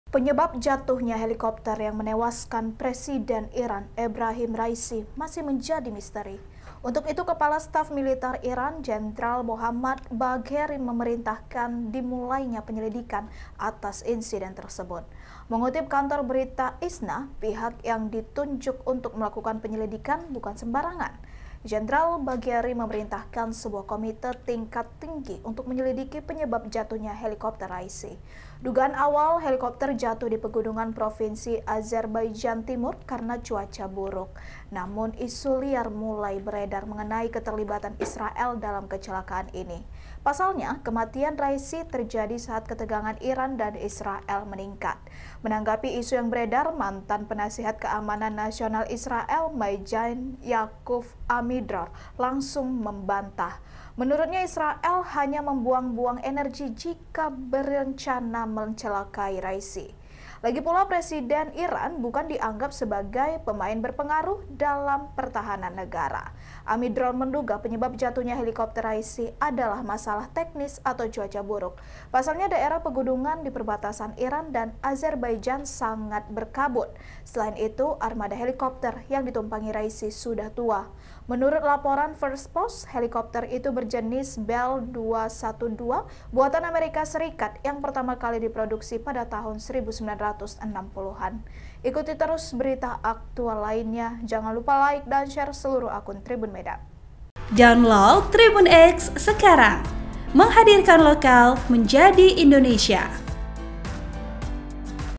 KOMANDAN IRAN GERAK CEPAT Cari Tahu Penyebab Jatuhnya Helikopter Raisi Meski Ada Isu Israel Terlibat
TRIBUN-MEDAN.COM - Penyebab jatuhnya helikopter yang menewaskan Presiden Iran Ebrahim Raisi masih menjadi misteri.

Untuk itu, Kepala Staf Militer Iran Jenderal Mohammad Bagheri memerintahkan dimulainya penyelidikan atas insiden tersebut.

Mengutip kantor berita ISNA, pihak yang ditunjuk untuk melakukan penyelidikan bukan sembarangan.

Jenderal Bagheri memerintahkan sebuah komite tingkat tinggu untuk menyelidiki penyebab jatuhnya helikopter Raisi.

Dugaan awal, helikopter jatuh di pegunungan Provinsi Azerbaijan Timur karena cuaca buruk.

Namun isu liar mulai beredar mengenai keterlibatan Israel dalam kecelakaan tersebut.

Pasalnya, kematian Raisi terjadi saat ketegangan Iran dan Israel meningkat.

Menanggapi isu yang beredar, mantan Penasihat Keamanan Nasional Israel (Mayjen) Yakoov Amidror langsung membantah.

Menurutnya, Israel hanya membuang-buang energi jika berencana mencelakai Raisi.

Lagi pula, Presiden Iran bukan dianggap sebagai pemain berpengaruh dalam pertahanan negara.

Amidror menduga, penyebab jatuhnya helikopter Raisi adalah masalah teknis atau cuaca buruk.

Pasalnya, daerah pegunungan di perbatasan Iran dan Azerbaijan sangat berkabut.

Selain itu, armada helikopter yang ditumpangi Raisi sudah tua.

Menurut laporan First Post, helikopter itu berjenis Bell 212 buatan Amerika Serikat (AS) yang pertama kali diproduksi pada tahun 1960-an.